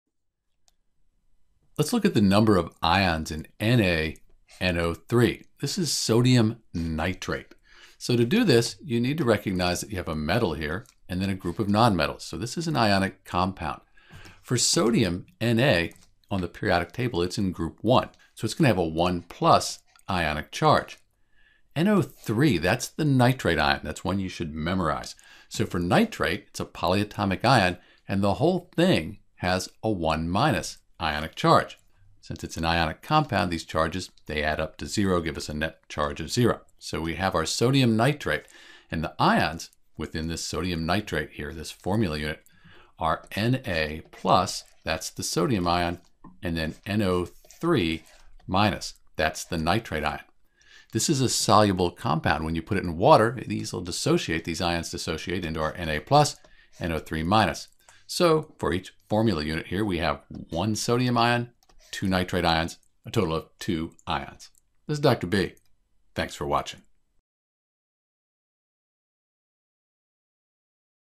Number of Ions in NaNO3 : Sodium nitrate
To determine the number of ions in NaNO3 you need to recognize the Sodium ion (Na+) and the Nitrate ion (NO3 -).

You can either memorize the polyatomic ions or, if you are allowed, you can look it up on a table of polyatomic ions

For Sodium nitrate there is one Na+  ion and one NO3 - ion.  So there are a total of two ions for one formula unit of NaNO3.

If you were given one mole of NaNO3 you would have1 mole of Na+ ions and 1 mole of NO3 - ions.